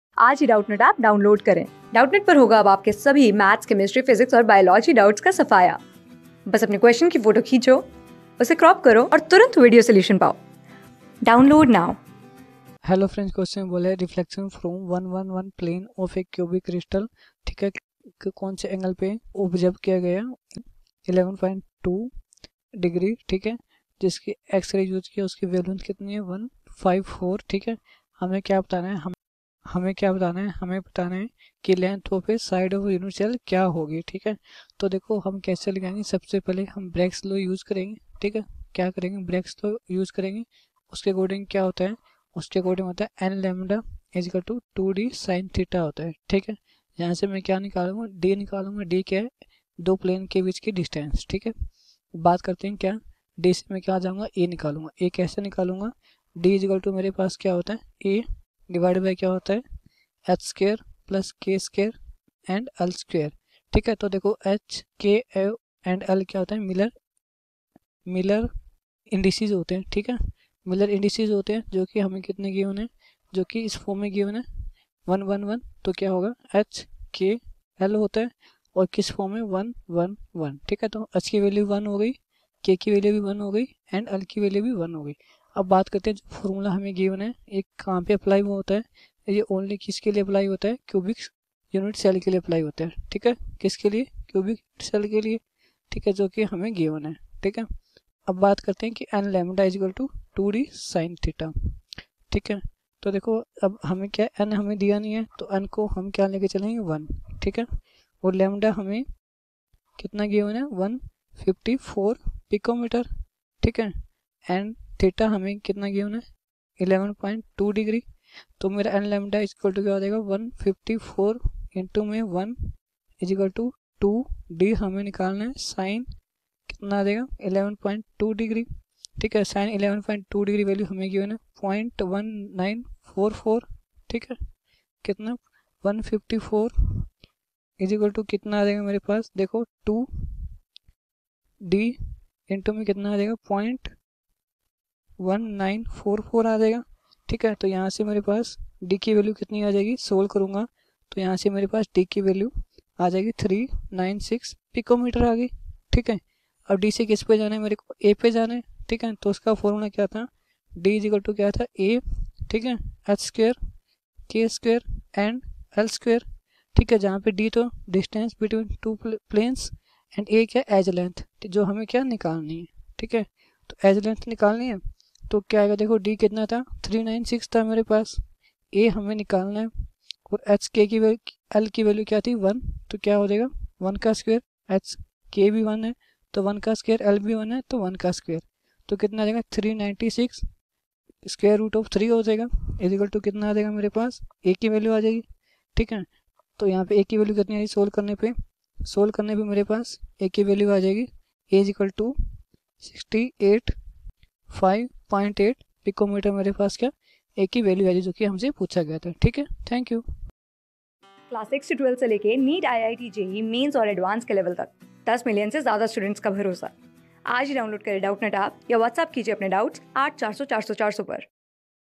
A reflaction from (111) planes of a cubic crystal was observed ad at a glancing angle of 11.2^(...
A reflaction from (111) planes of a cubic crystal was observed ad at a glancing angle of  11.2^(@)when X -raysof wavelength154 pm were used. What is the lengthof the side of the unit cell ?( sin 11.2^(@) = 0.1944)
Class: 11
Subject: CHEMISTRY
Chapter: SOLID STATE
Board:CBSE

👉 Have Any Query? Ask Us.